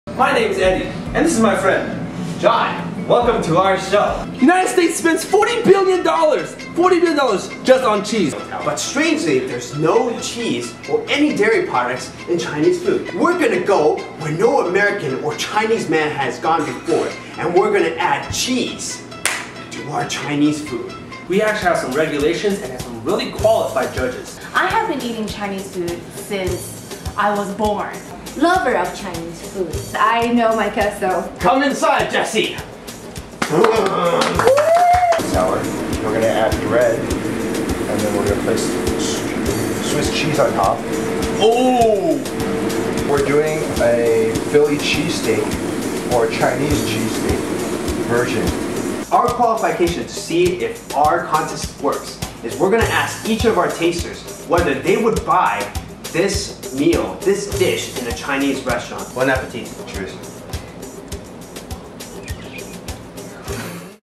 The Yellow Cow Ep. 1 Trailer #2
"The Yellow Cow" is a food show that fuses the American and Chinese cultures by adding an American element to Chinese traditional dishes. This is only a minute trailer to show you what went on. Enjoy!